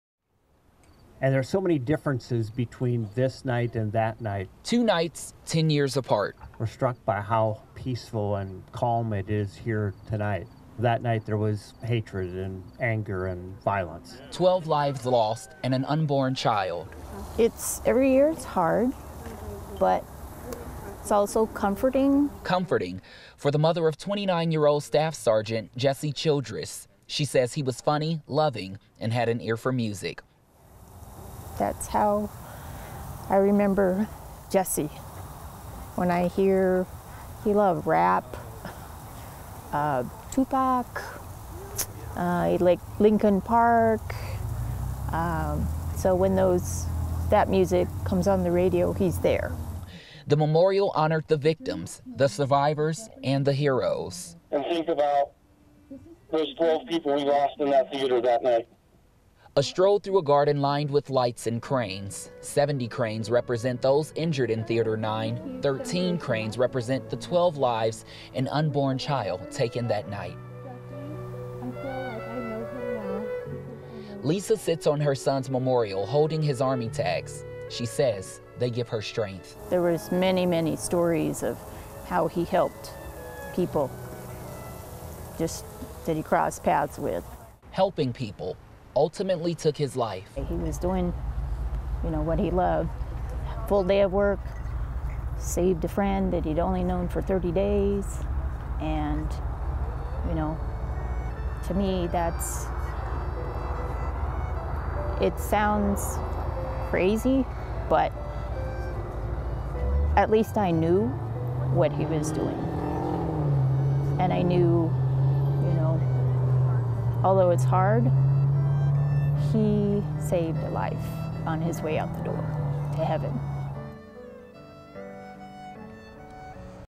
Overnight memorial remembers those of the Aurora Theater Shooting 10 years later
7/20 Memorial Foundation hosts 10 year memorial honoring the victims, survivors, and heroes of the Aurora Theater Shooting in 2012.

9NEWS (KUSA) is located in Denver, Colorado.